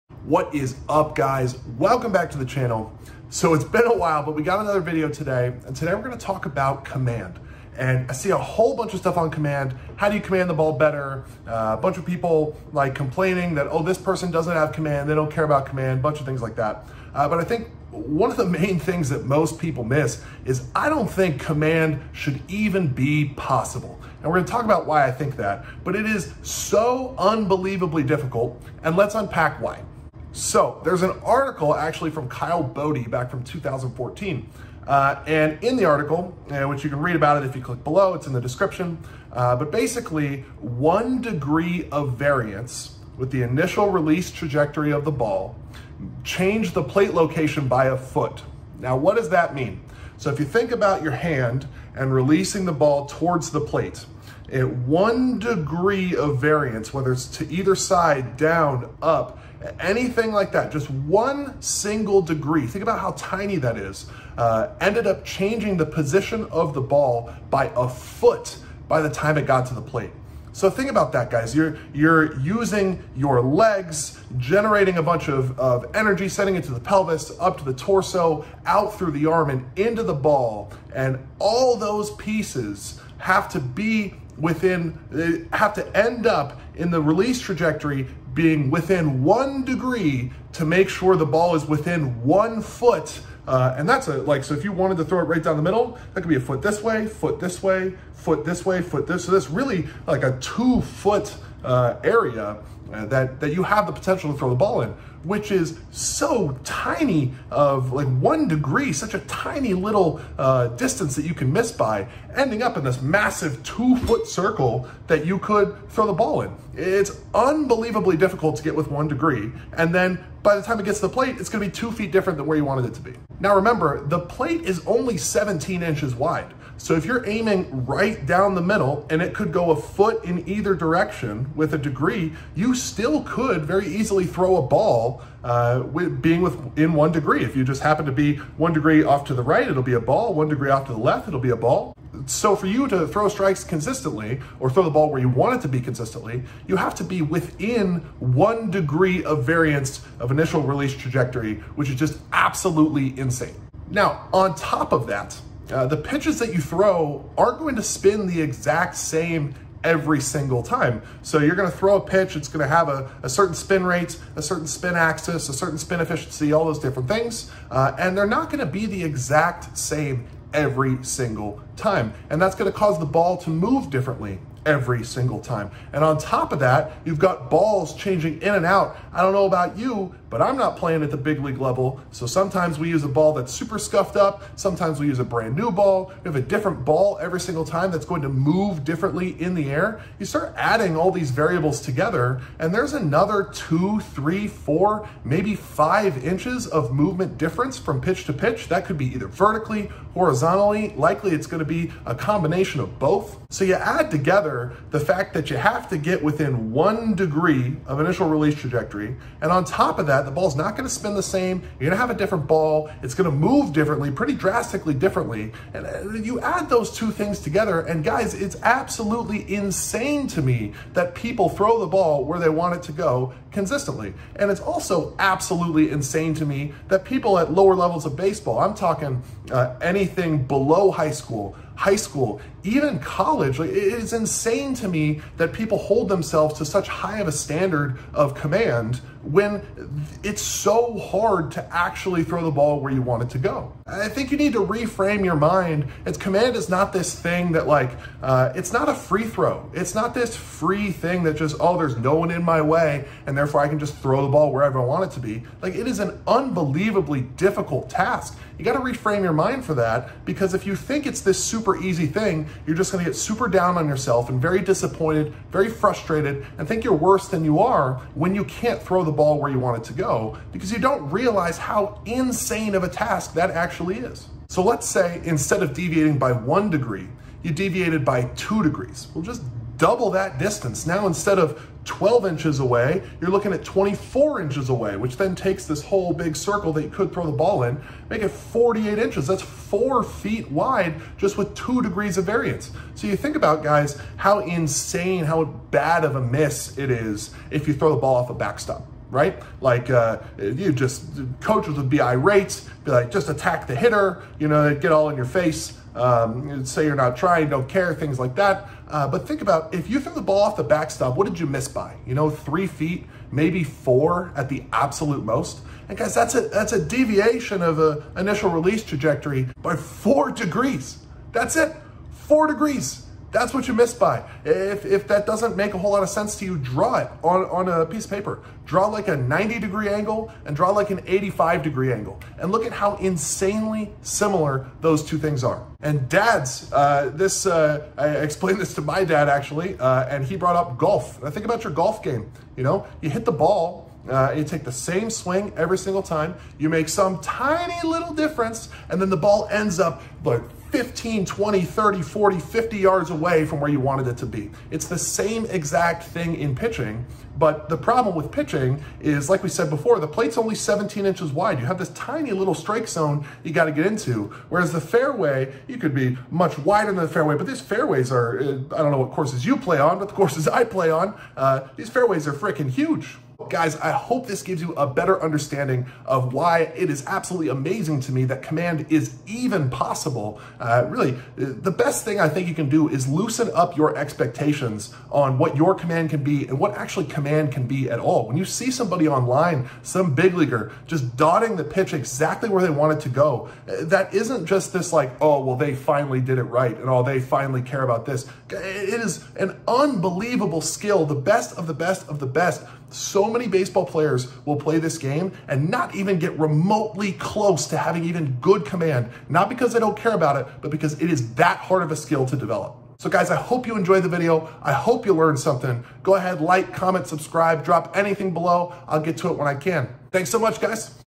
Pitching Command Should NOT be Possible
Throwing the ball where you want it to go while pitching is an unbelievably difficult task. So insanely difficult that I am amazed it's actually even possible. Let's talk about why